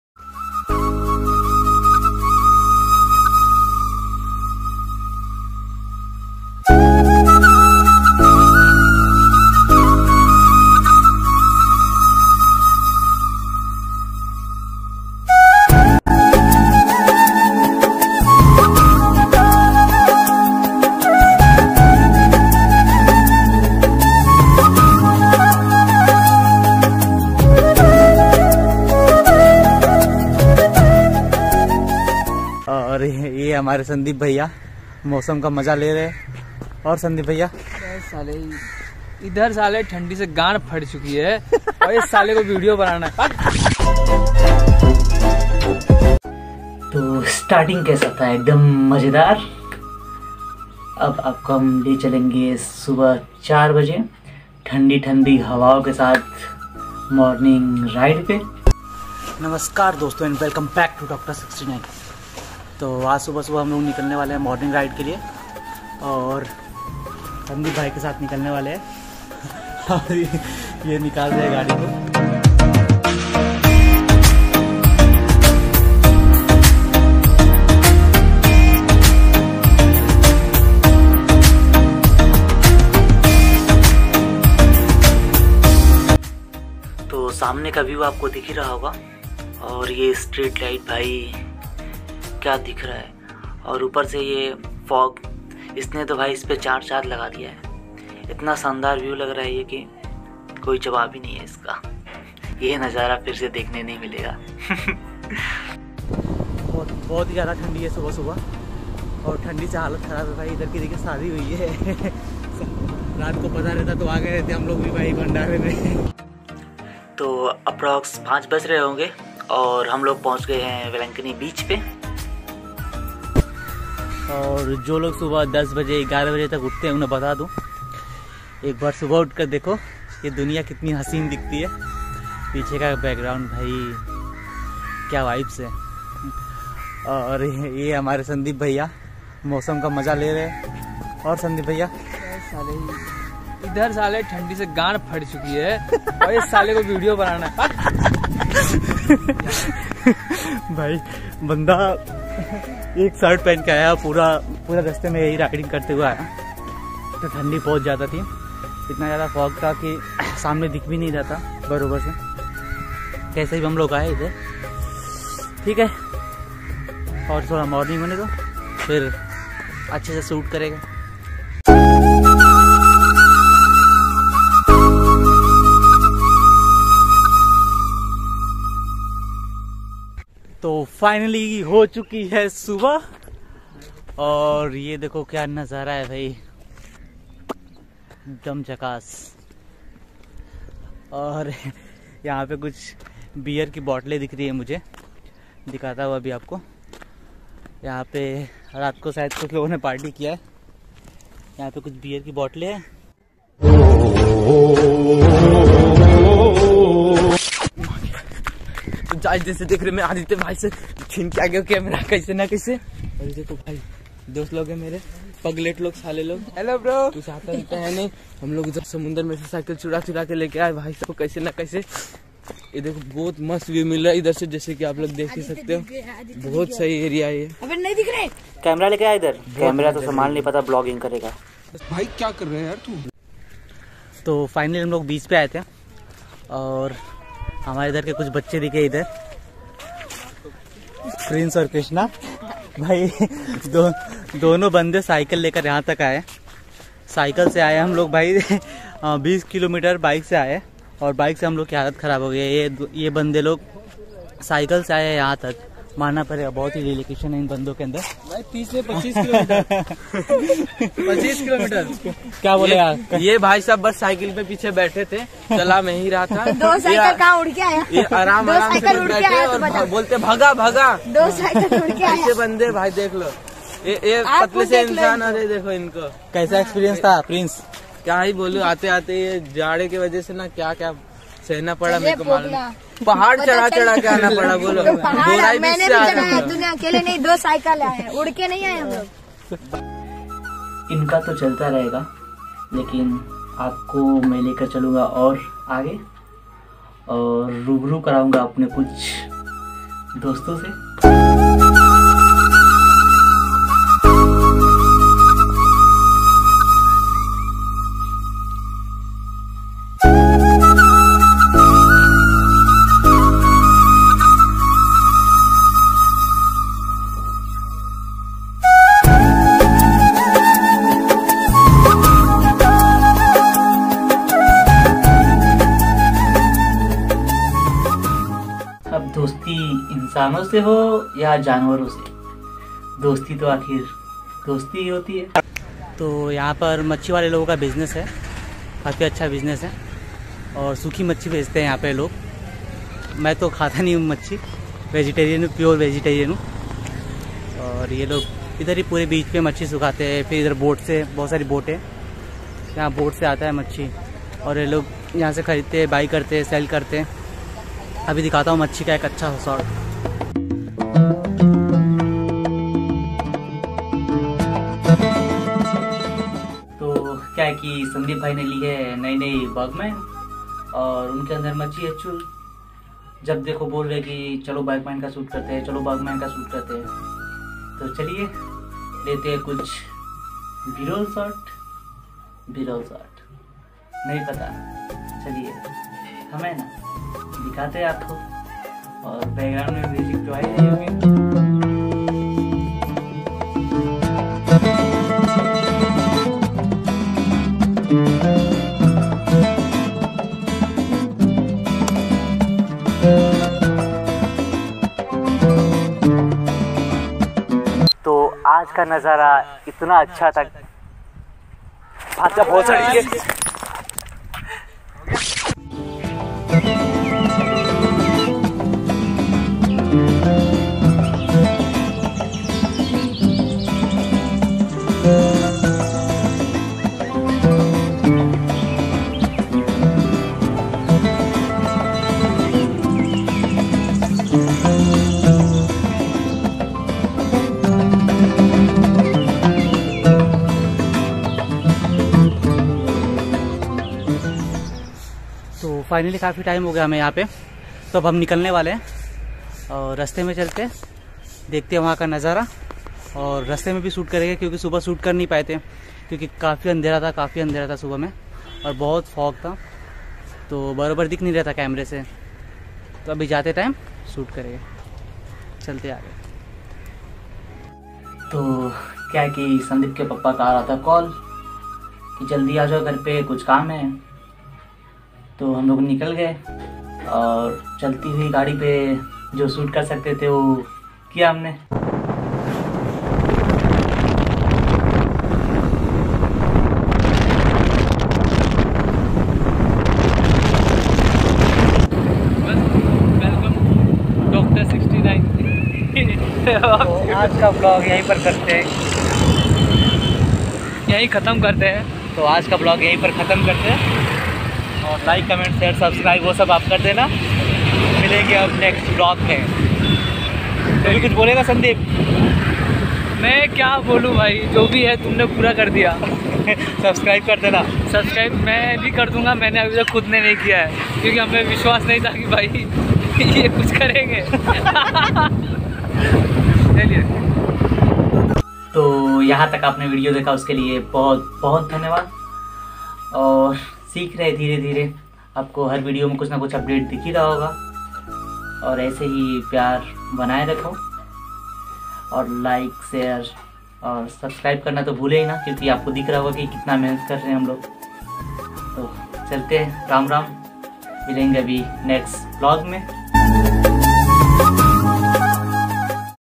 Morning Ride | Bhayandar to Velankanni Beach | Vlog | CINEMATOGRAPHY
Toh Aaj ke vlog me aap ko EMOTION , FUN , FRIENDSHIP , CINEMATOGRAPHY , NATURAL BEAUTY sab kuchh milega...